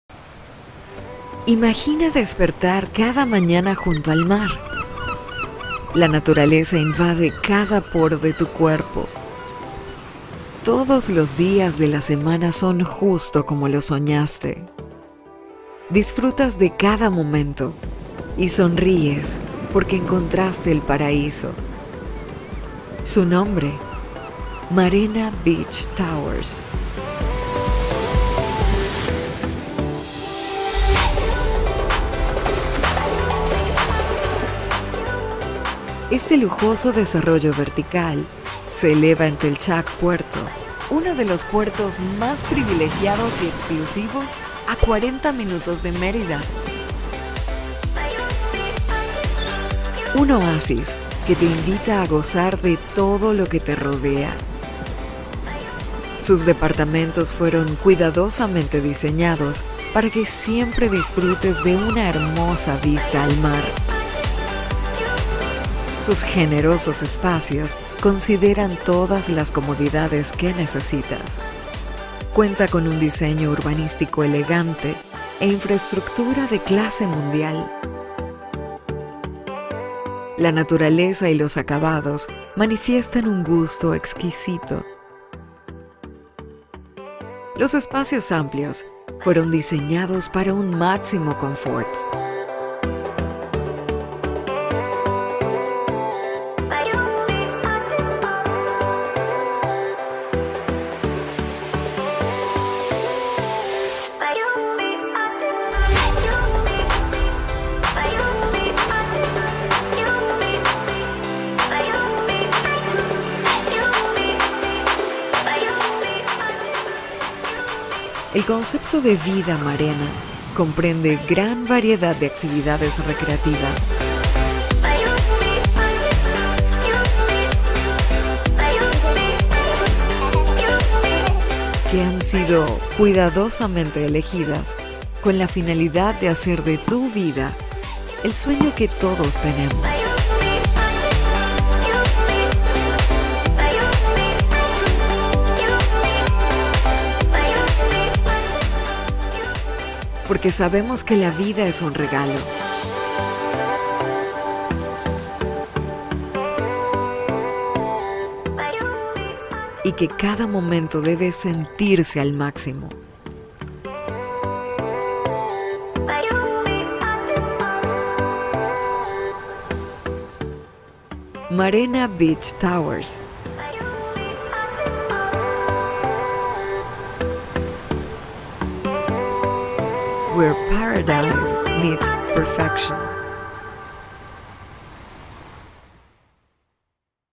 INCREIBLES DEPARTAMENTOS A LA ORILLA DE LA PLAYA EN TELCHAC - MARENA BEACH TOWERS
Marena es un exquisito desarrollo vertical creado con los más altos estándares de calidad para ofrecer un producto residencial premium.

Con una arquitectura vanguardista, Marena ha sido estratégicamente diseñado para que todos los residentes diusfruten de vistas privilegiadas, denotando un extraordinario trabajo de planeación.

La exclusividad se refleja en sus acabados de lujo, el concepto de vida y su imponente vista frente al mar; además de una composición vertical escalonada que se convierte en el distintivo de esta joya arquitectónica y que se ubica en el Puerto de Telchac, en el estado de Yucatán.